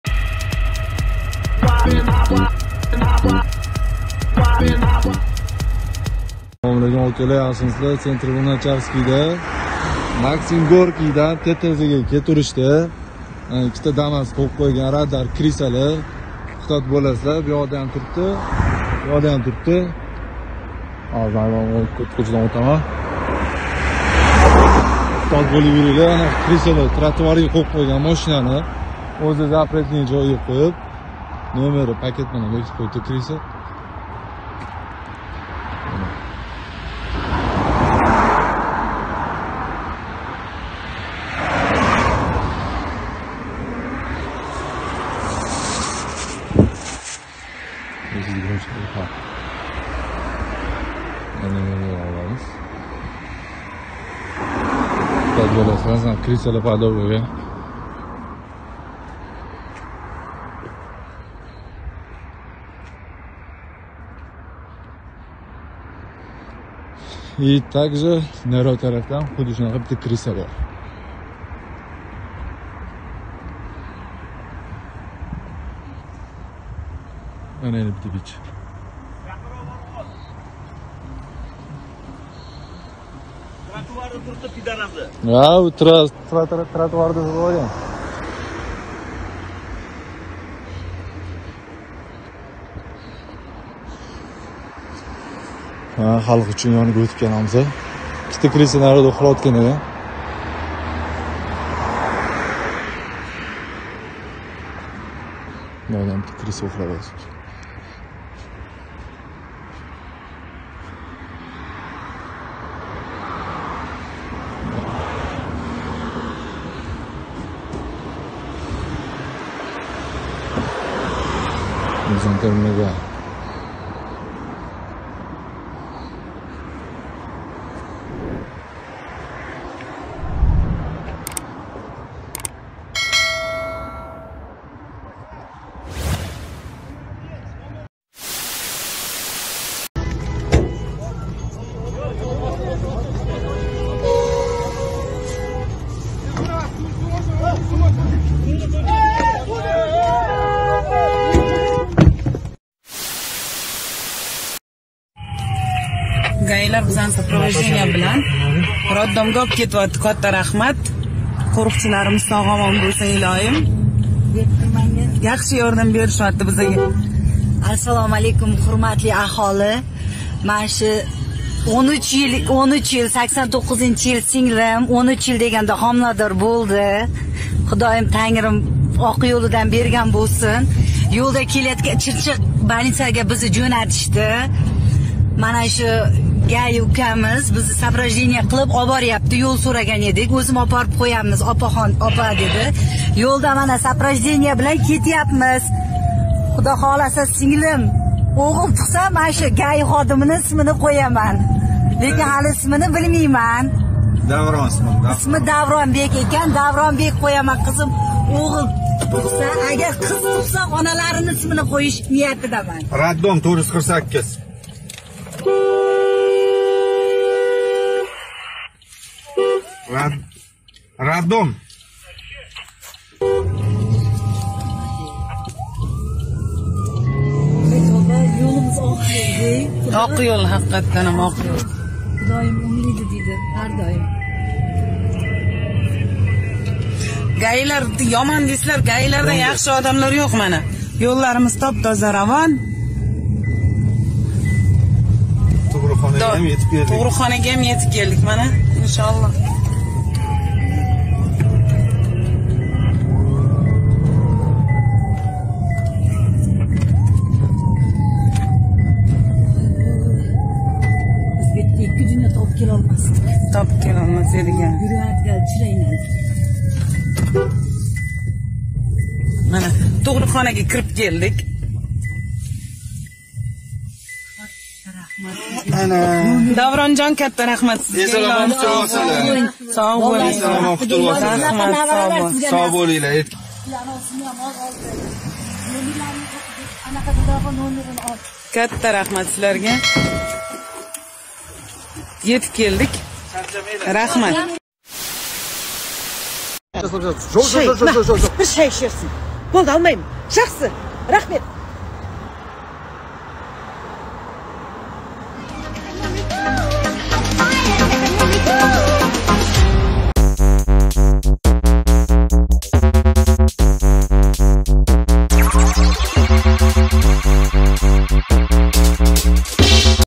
ГАИ ва яширин радарлар
00:00 Эҳтиёт бўлинглар, яширин радарлар
02:18 Қашқадарёда ГАИ машинаси ёниб кетди. Гаи ва ИИБ ходимлари оловни ўчиришга харакат қилмоқда
02:33 "13 йил деган фарзанд кўрмоқда қизим, ўғил туғса шу ГАИ боламни, қиз туғса оналарини исмини қўяман набирамга".
Тошкент вилояти ЙПХ ходимлари туғруққа кетаётган аёлни раддомга ўз вақтида етказишди.
04:55 "Гаидан қутулиш" - прикол